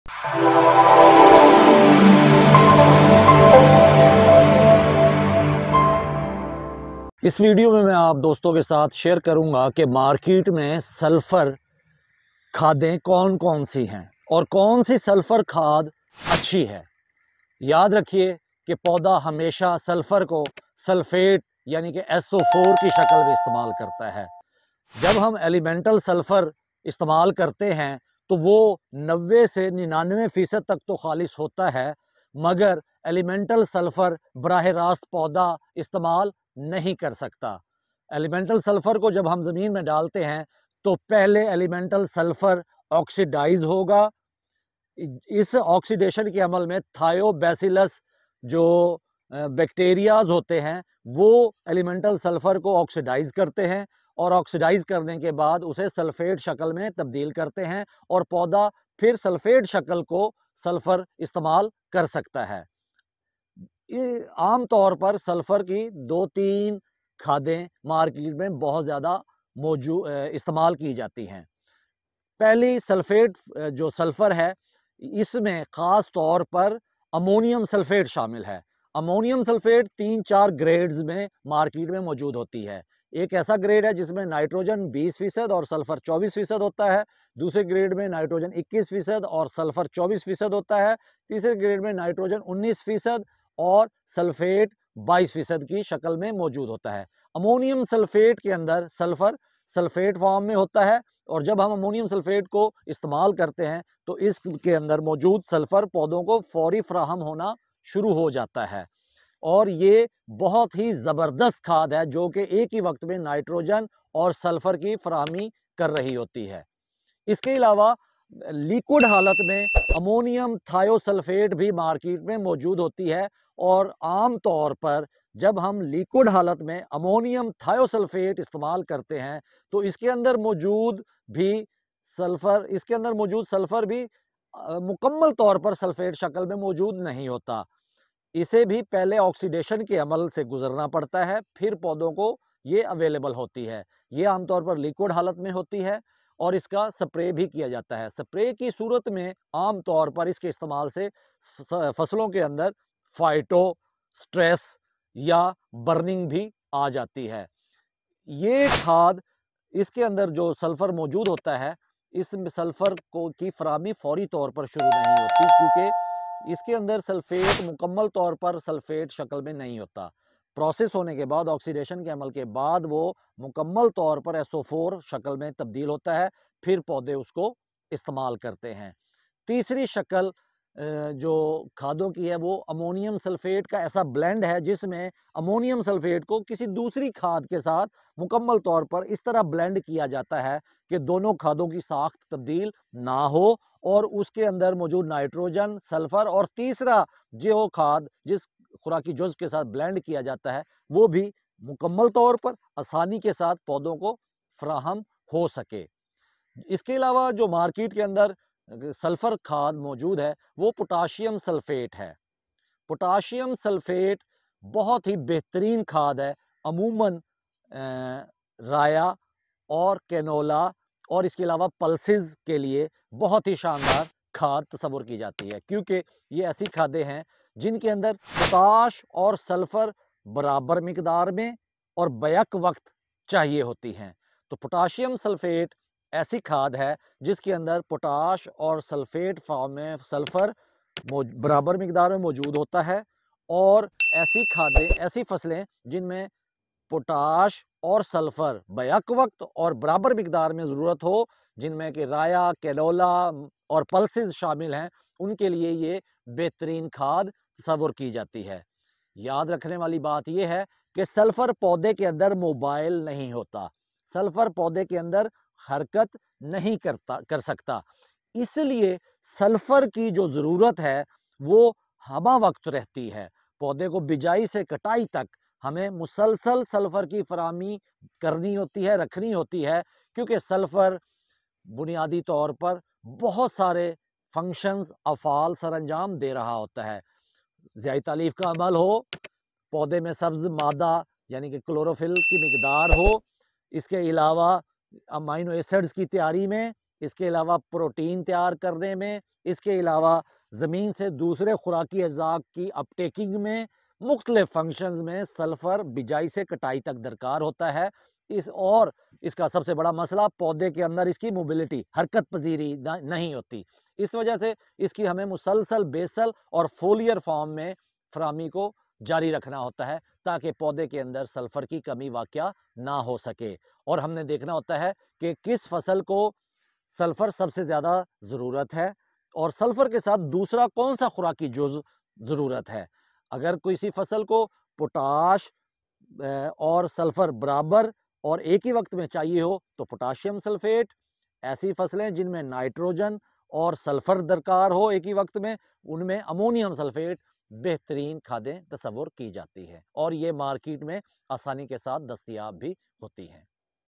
Sulfur based Fertilizers
In this informative video, fertilizers having sulfur, are explained.
It is also elaborated that, which sulfur based fertilizer is better for which crop. If we know about it, we can save money and can have better results from these fertilizers